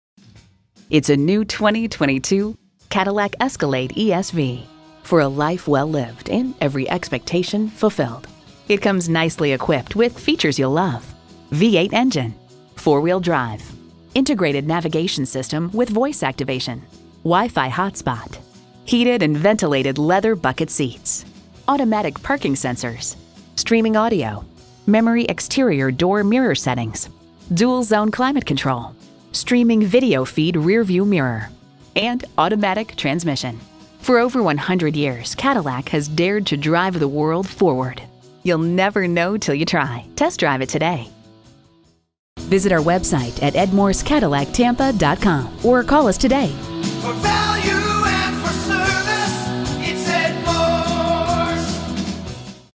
New 2022 Cadillac Escalade ESV Tampa FL St. Petersburg, FL NR162262 - SOLD
Year: 2022
Make: Cadillac
Model: Escalade ESV
Trim: Premium
Engine: 6.2l 8 Cylinder Fuel Injected
Transmission: 4WD 10-Speed Automatic with Overdrive
Color: Black Raven
Interior: Black
Stock #: NR162262
VIN: 1GYS4KKL8NR162262

Address:
101 East Fletcher Avenue